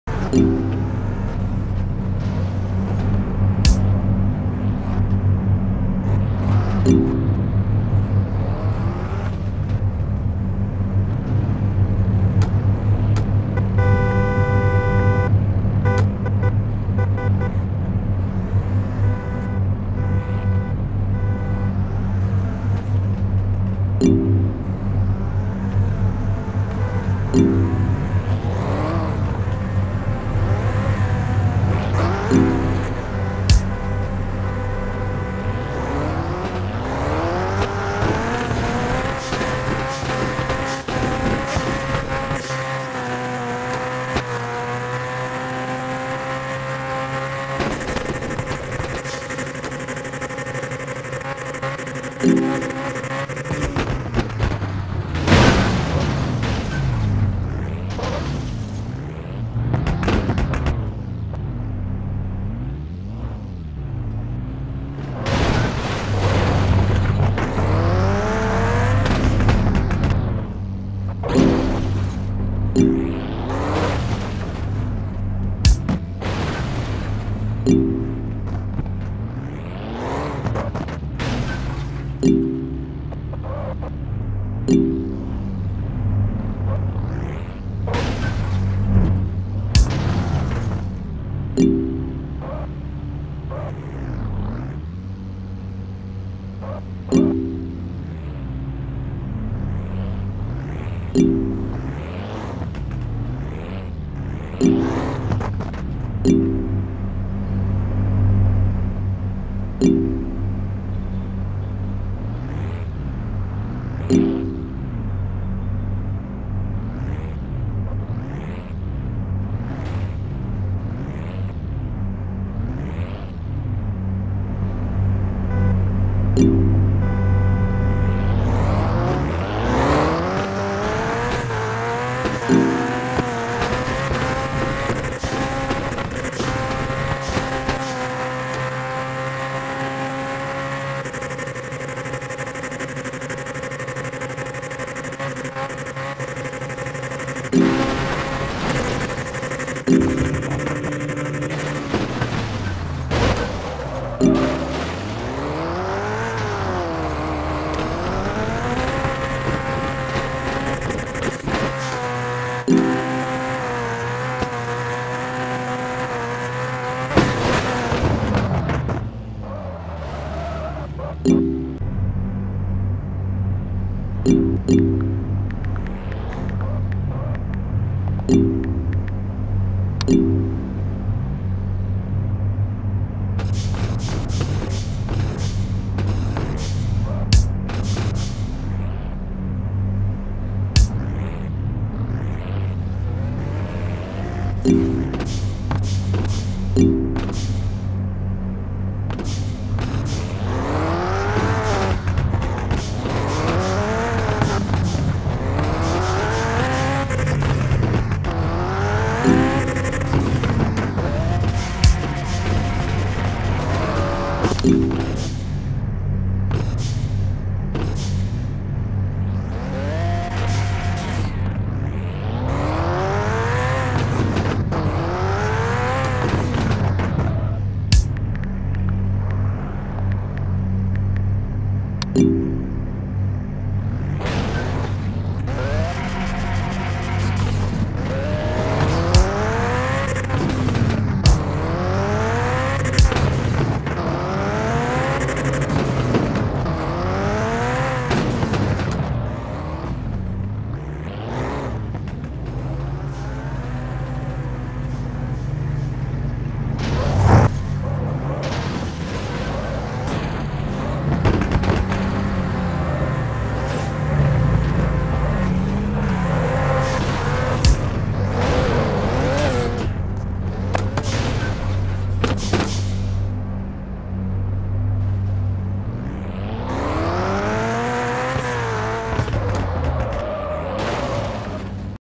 HOW MANY PAGANI HUAYRA WHERE THERE
Y does this car take flight like that I just got this car in it spent more time in the air then on the road